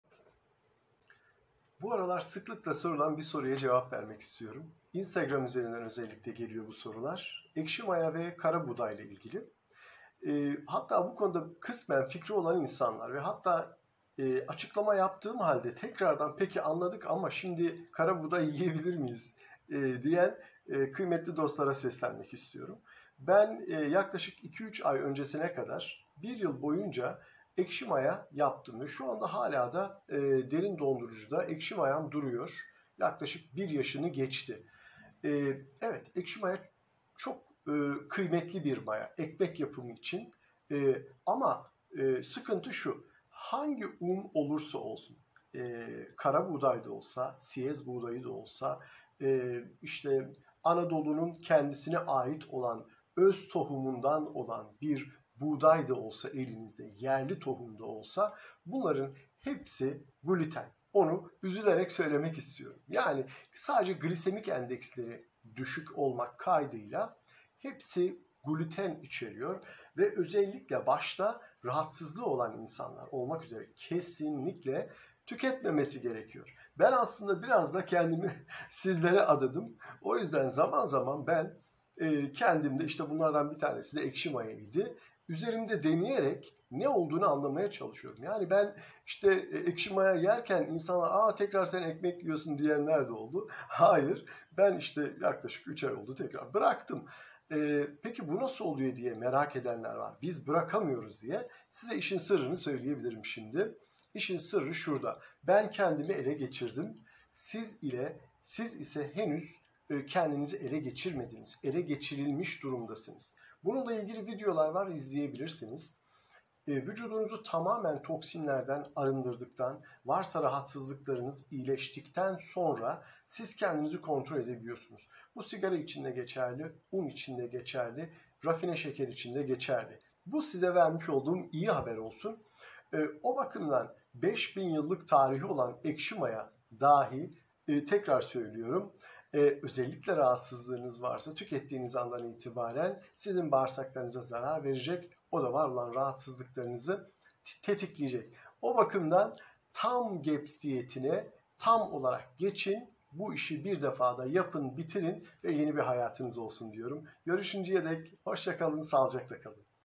GAPS Ekşi Maya Nedir Ne Değildir?
* Ekşi maya nedir?
* Neden ekşi maya?
* Gluten nedir?
* Glisemik endeks nedir?
* Neden un tüketmemeliyiz?
* GDO nedir?
* 100 yıl önce GDO var mıydı?
* Ne yediğinizi biliyor musunuz?
* Un neden ödem yapar?
* Ödem enflamasyon yapar mı?
* Enflamasyon neden endokrini bozar?
* Endokrin nedir?
* Endokrin bozulursa hormonlar üretilse bile neden yerine ulaşamaz?
* Ekşi maya kötünün iyisi mi?
* 100 yıl öncesinin insanı ile günümüz insanı aynı "gıdaları mı" tüketiyor?
* Artık hem hayatta kalmak hemde sağlıklı kalmak için tek bir şansımız var!
* FABRİKA AYARLARIMIZA dönmek zorundayız!
🍀 Yaşamak için ikinci bir şansımız var mı? O zaman.. düşünmeyin! Başlayın! 🌈 Kendinize geldiğiniz de bol bol düşünebilirsiniz.